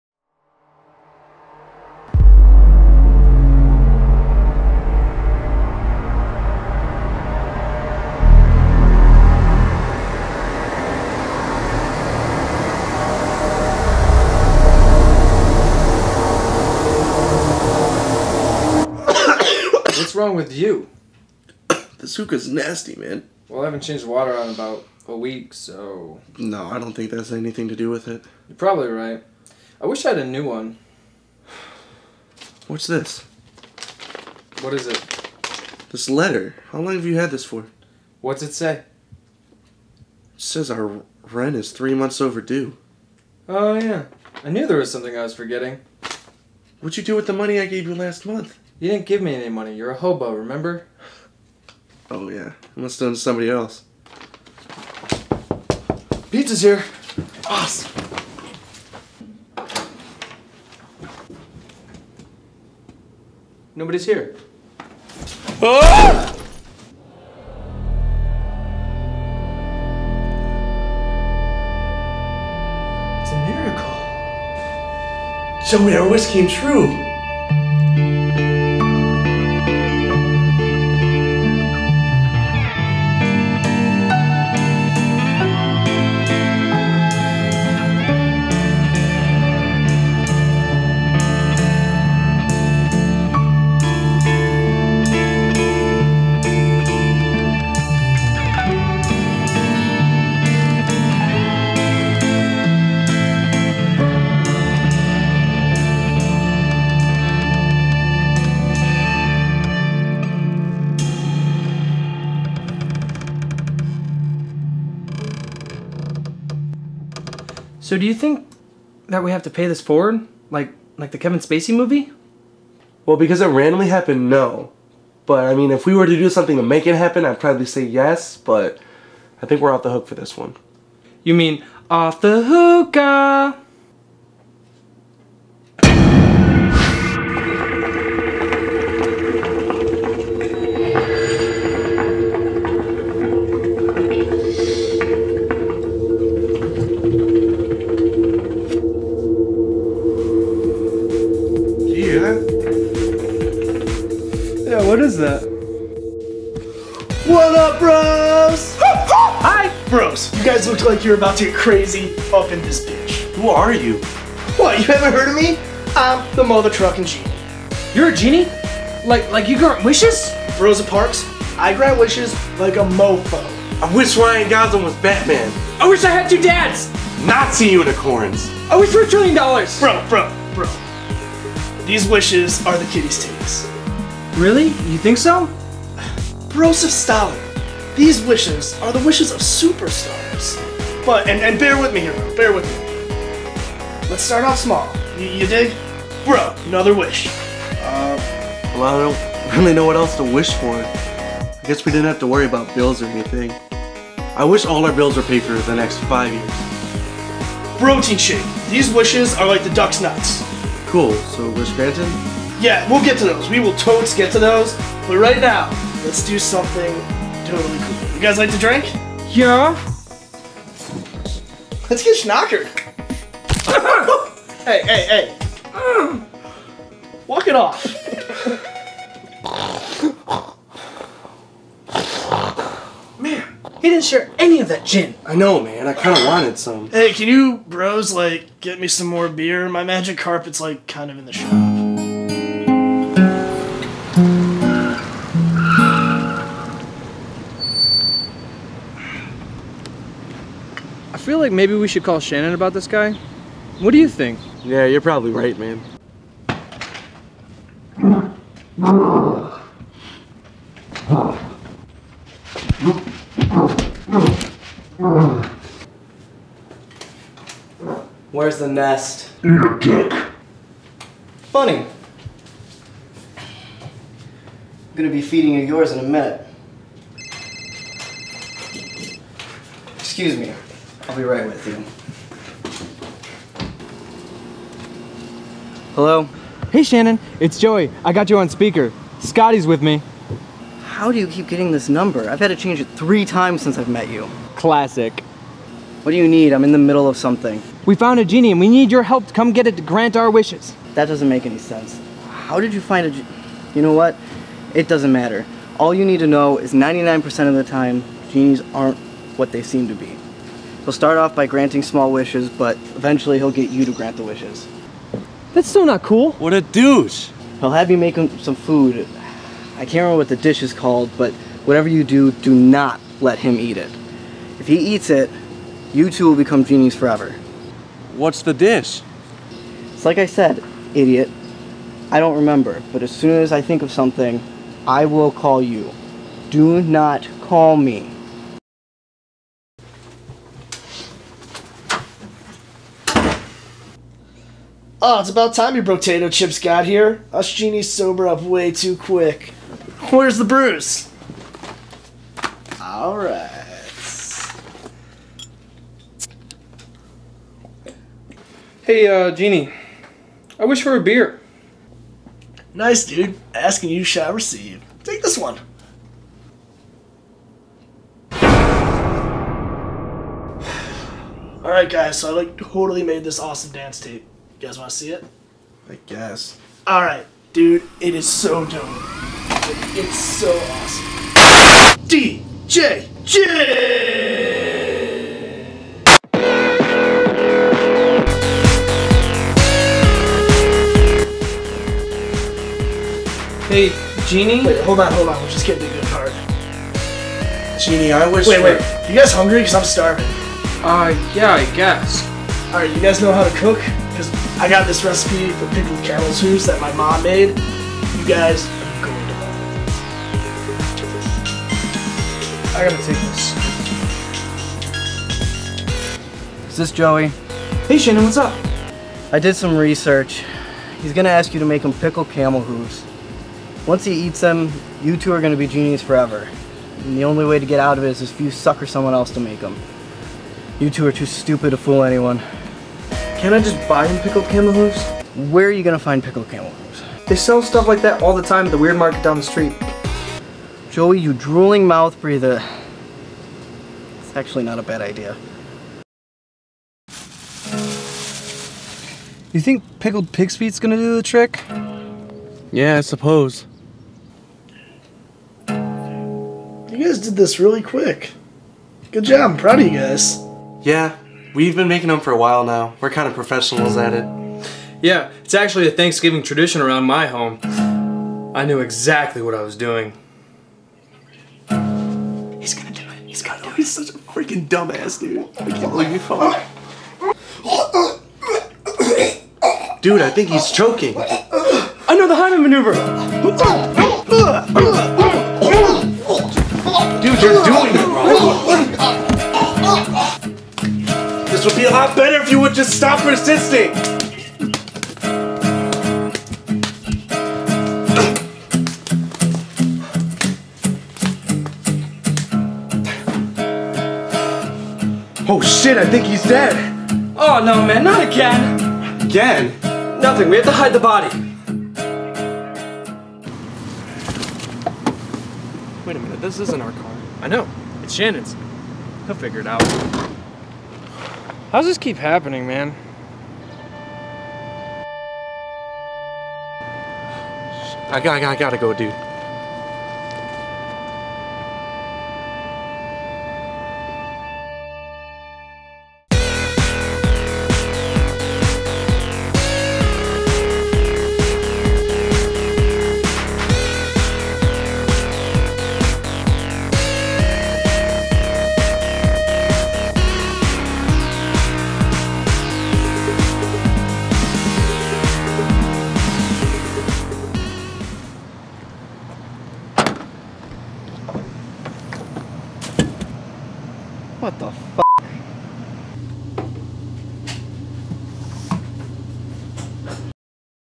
ROOM FOR RENT EP 5 - D.J. Genie in a Bottle
Do you ever wish that a genie would come and grant all of your wishes? Solve all of your problems? hang out and be a cool dude? Well Keep wishing!